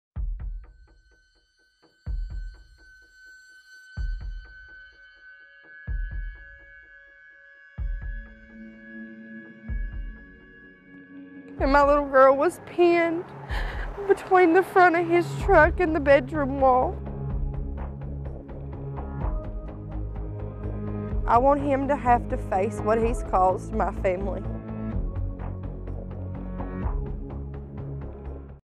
ICYMI- 6-year-old pinned to wall when truck crashes into Arkansas home
A man is facing charges after a truck crashed through an Arkansas home while a family was sleeping inside.